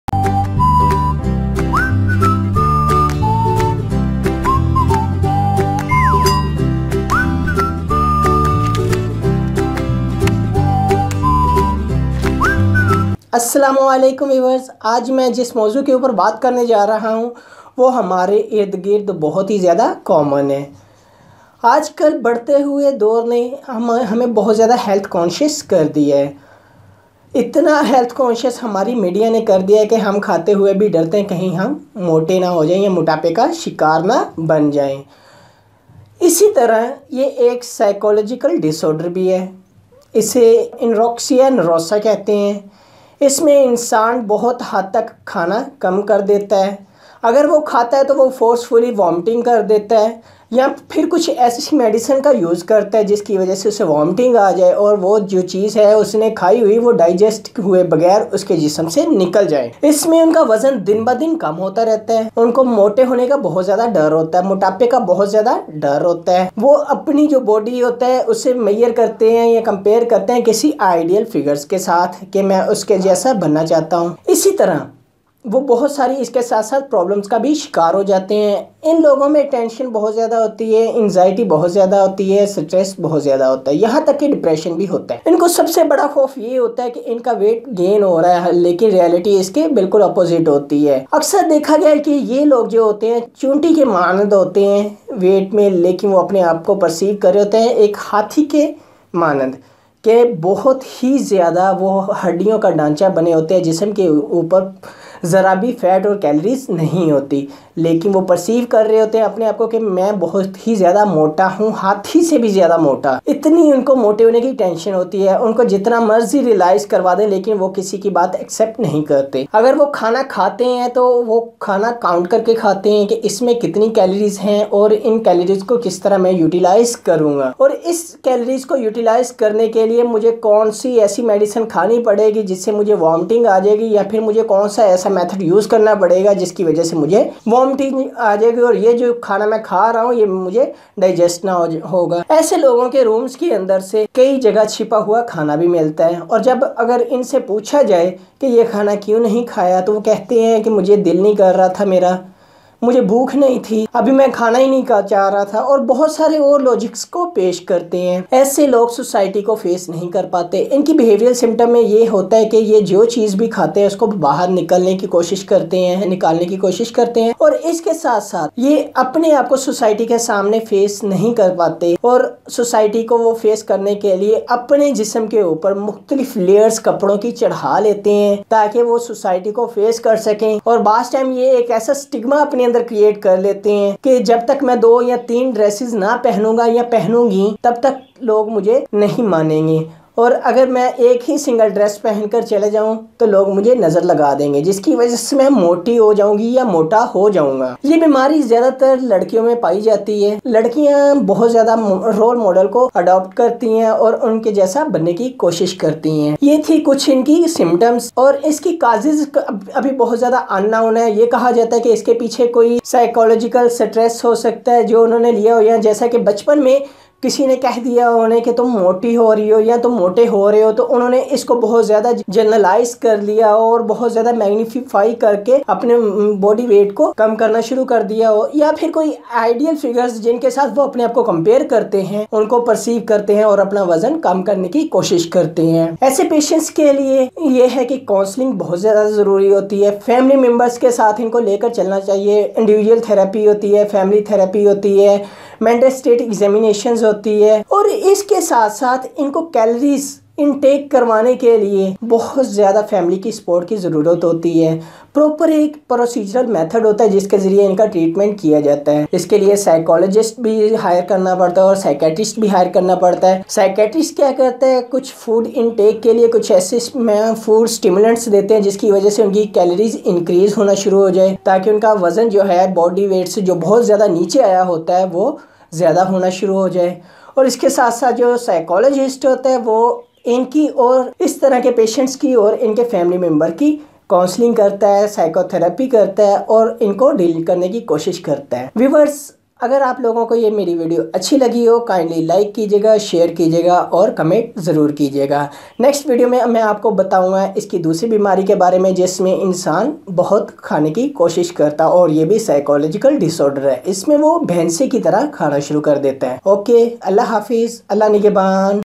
Anorexia Nervosa (Eating Disorder) in Urdu/Hindi by Hafeez Haider
This Video is regarding Eating Disorder (Anorexia Nervosa). Anorexia Nervosa is an eating disorder in females in a majority of cases where the weight of a patient is less than normal. Want to know more about anorexia nervosa...Keep Watching.